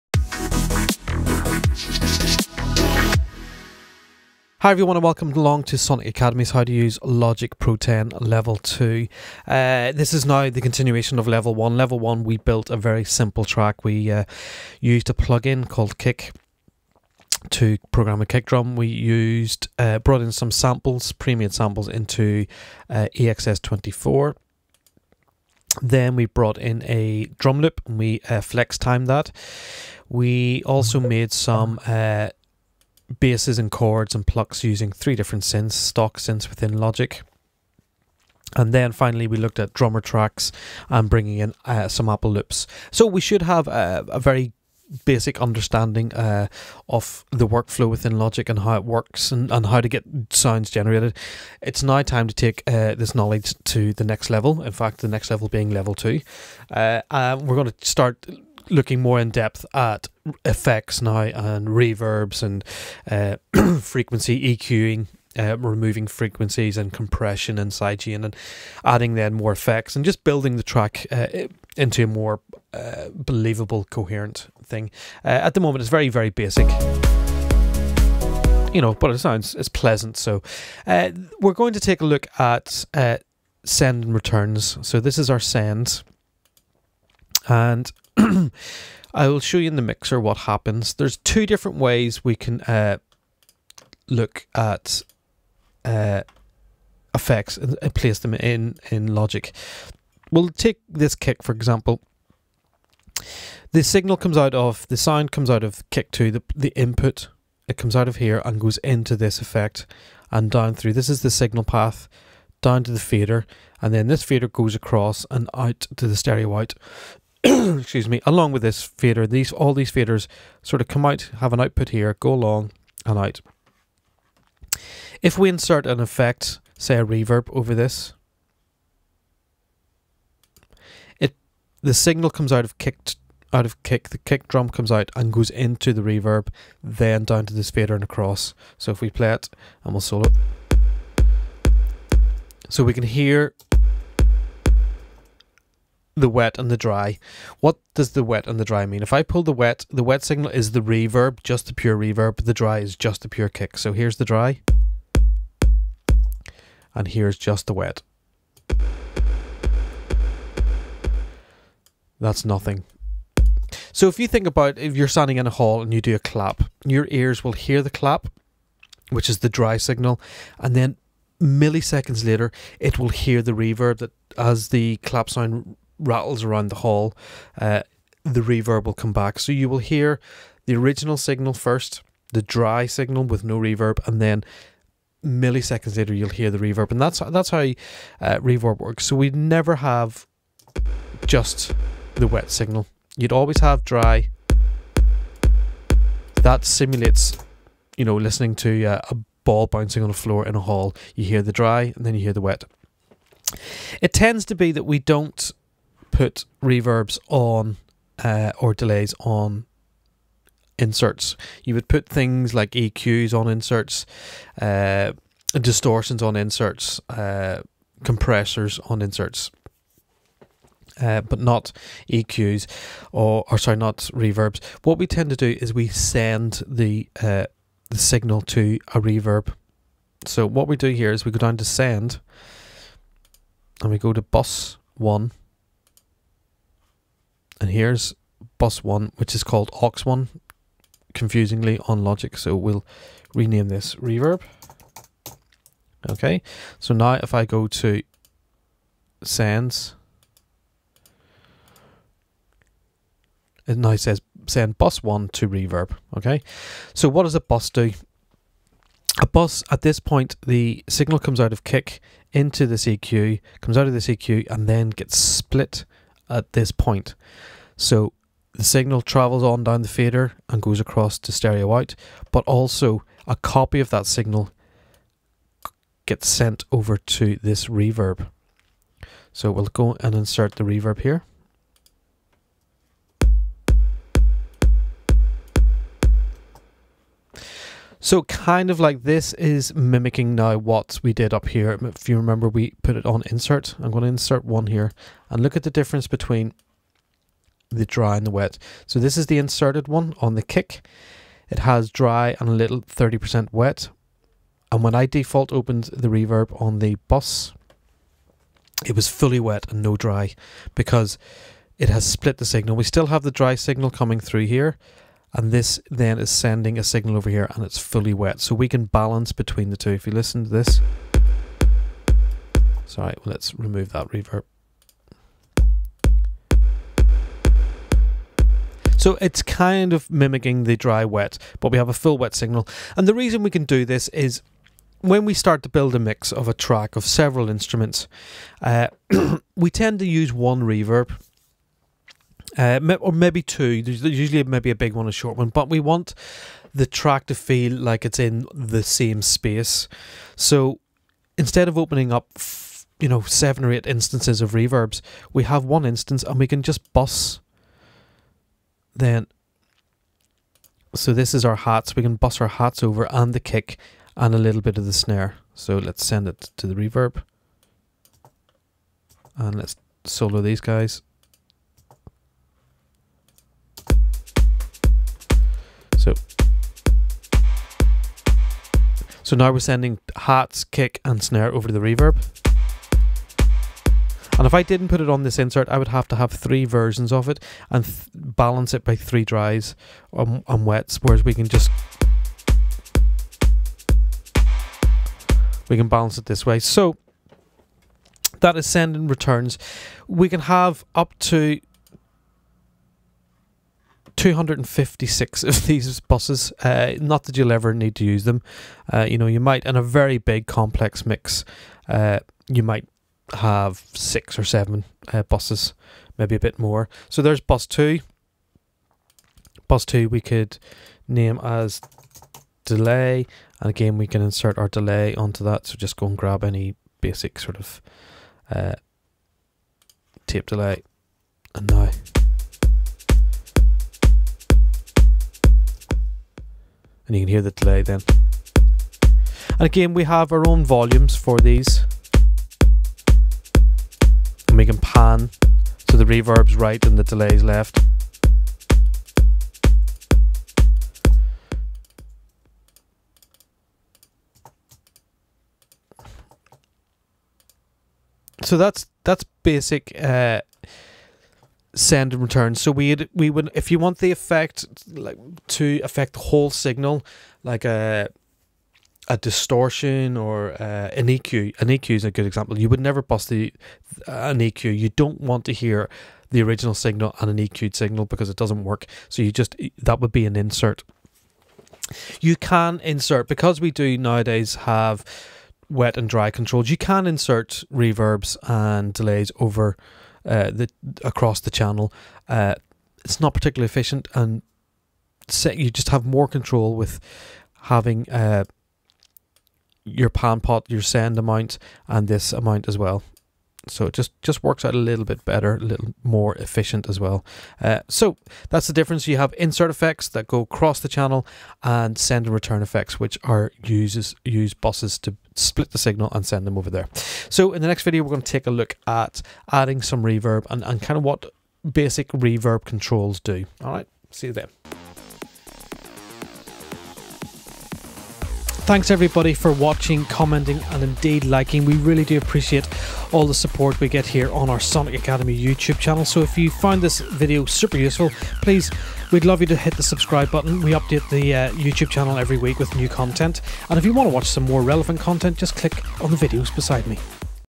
How To Use Logic Pro X - Beginner Level 2 - Send Return FX
Over the next 13 videos, Sonic Academy's very own Chris Agnelli dives further into Logic Pro X with a brand new set of tutorials. If you are a complete beginner or even a seasoned pro, this course has something for everyone.

How To Use Logic Pro X Level 2, continues on from where Level 1 finished off. It is quite literary NEXT LEVEL!

With loads of useful knowledge for producers starting out, we begin by checking out how to use send/return fx, taking a closer look at adding reverb and delay’s before moving on to best practices for EQ’ing, compression and that all important sidechain. Chris wraps up by adding more synths and touching on some mixing and mastering techniques to help make the track shine.

For those of you who have recently started out using Logic Pro X, you'll be armed and ready to start on your production journey, so go forth and create!